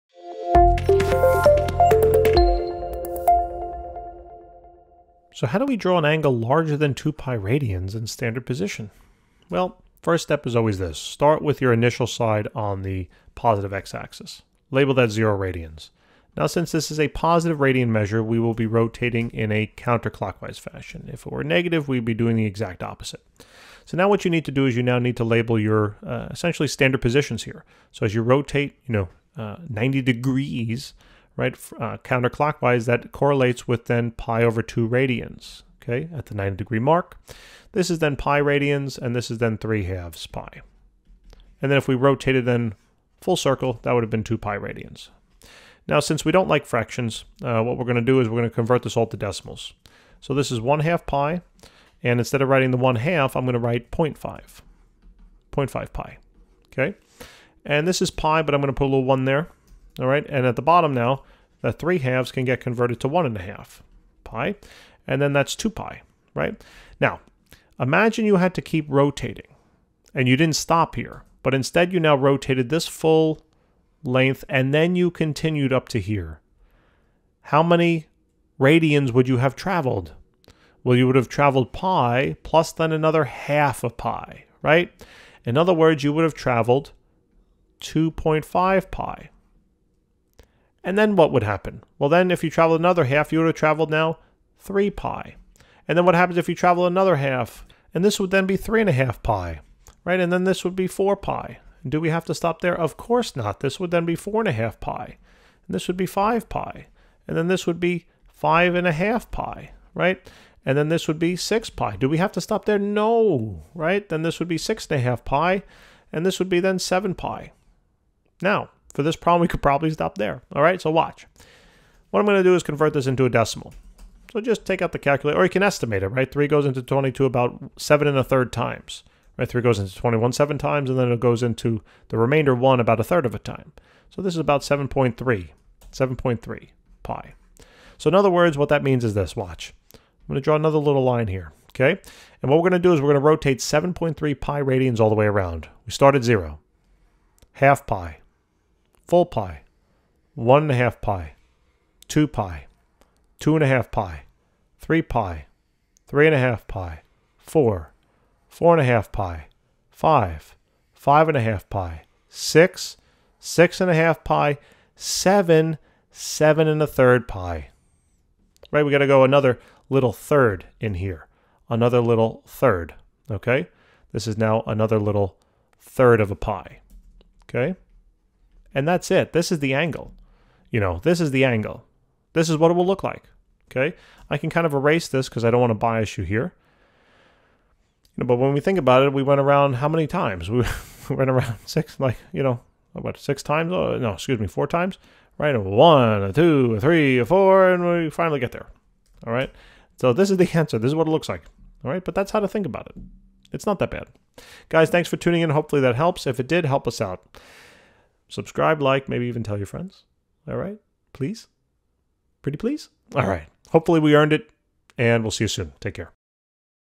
Draw 22/3 pi radians in standard position
For the following exercises, draw an angle in standard position with the given measure.
22π/3